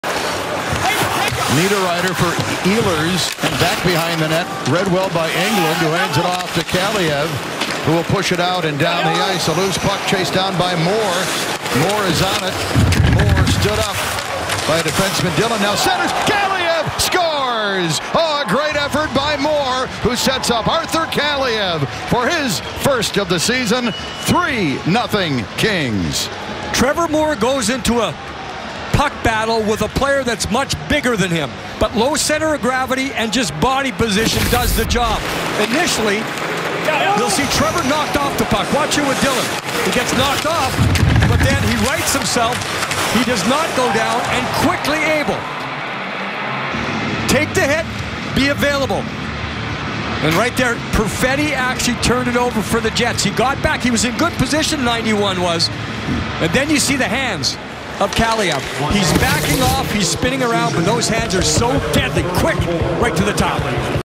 Trevor Moore wins the puck battle and sets up Arthur Kaliyev down low for a goal.
Goal:
Arthur Kaliyev (1)

Assist:
Trevor Moore (1)